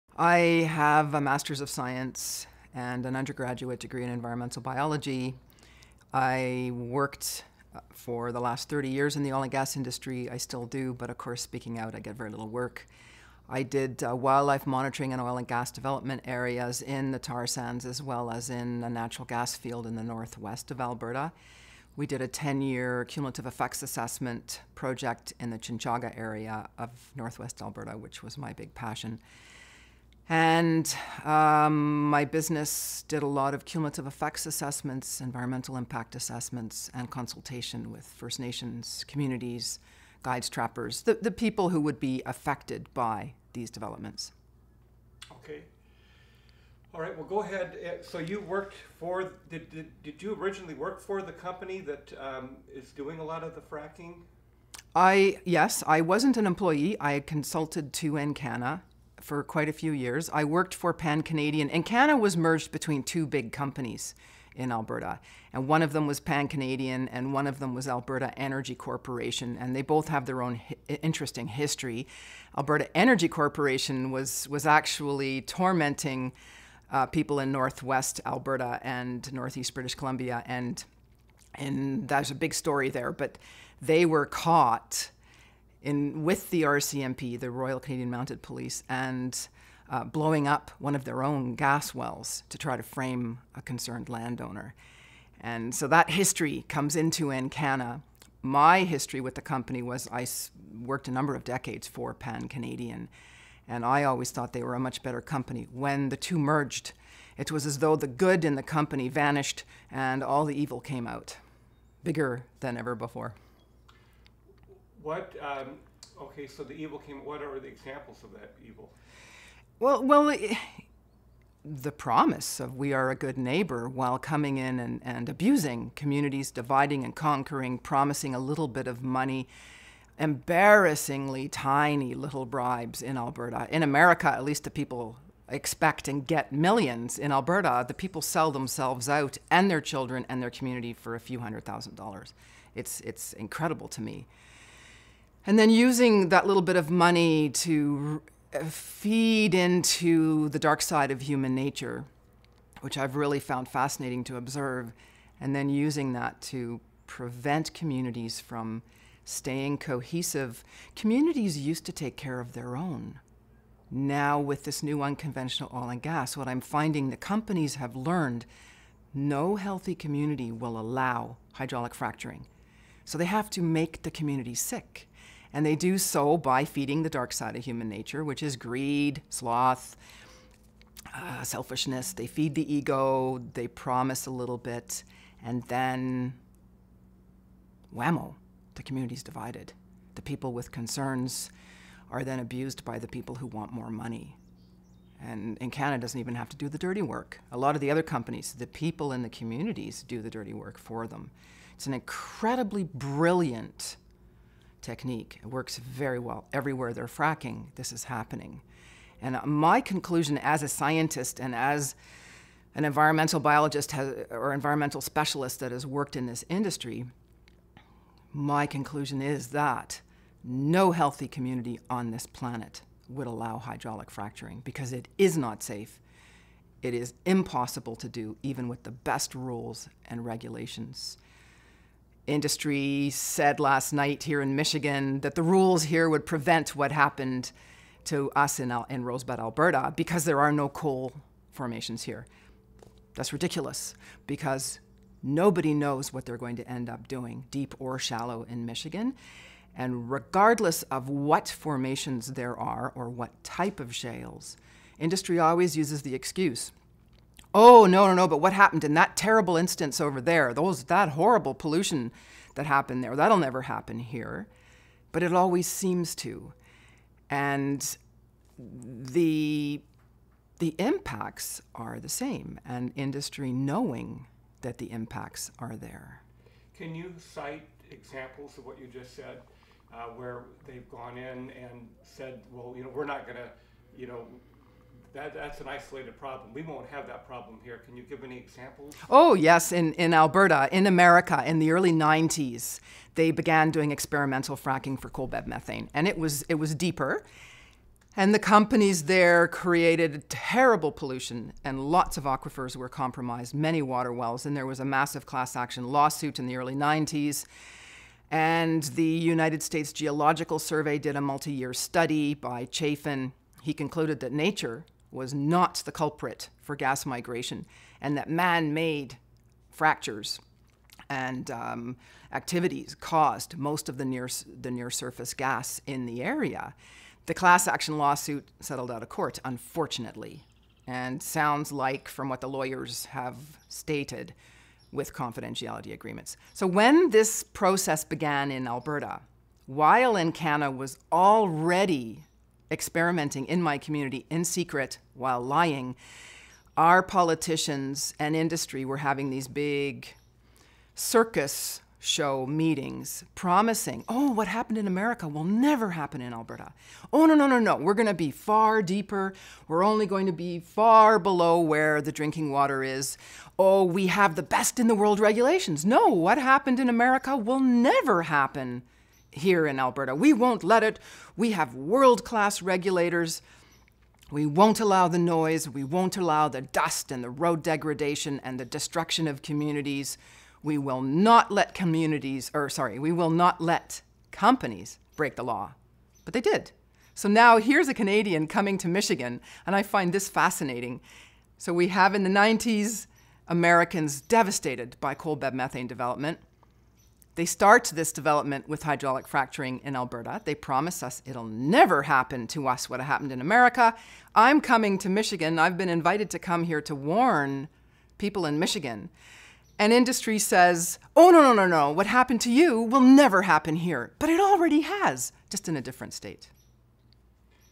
Jessica Ernst, The Consequences of Fracking
Jessica Ernst is a scientist who has worked in the oil and gas industry.  She discovered first hand the consequences of hydraulic fracturing in her town of Rosebud, Alberta, Canada.  Jessica has come to Michigan and to other places around the world to warn communities of the dangers of fracking.